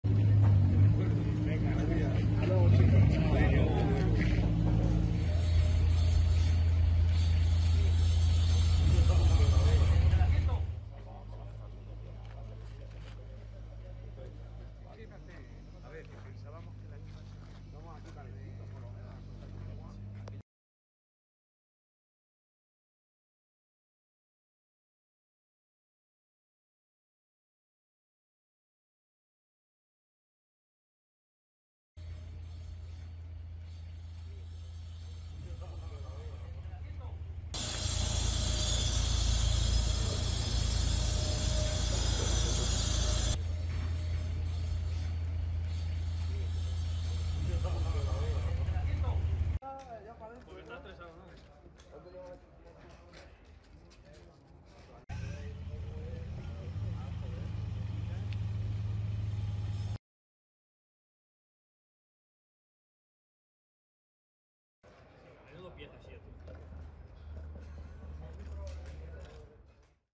Instalan rampas mecánicas en Ávila
Instalan rampas mecánicas en la plaza de Ajates. Ávila, 19 diciembere 2023.